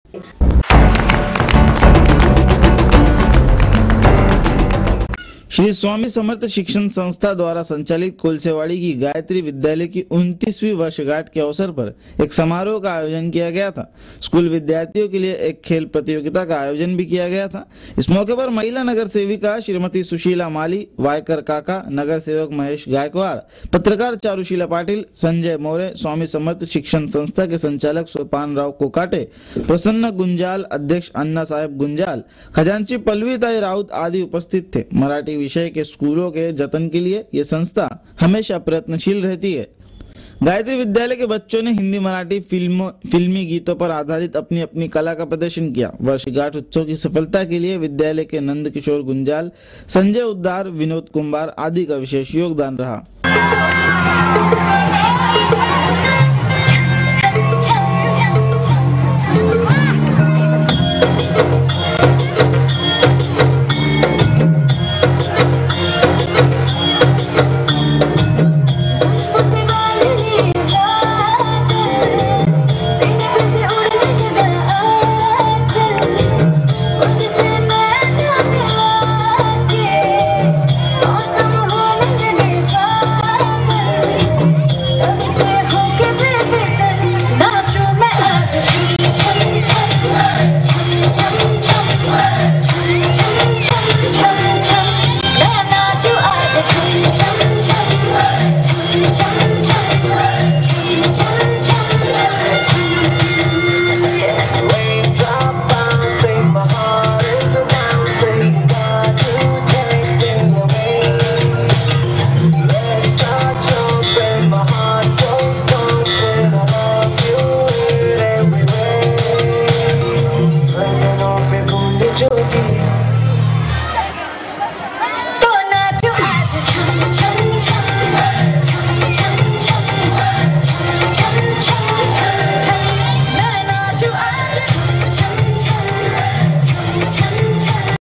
gayatri school x264
गायत्री विद्दयालय की २९ वी वर्षगांठ धूम धाम से मनाई गयी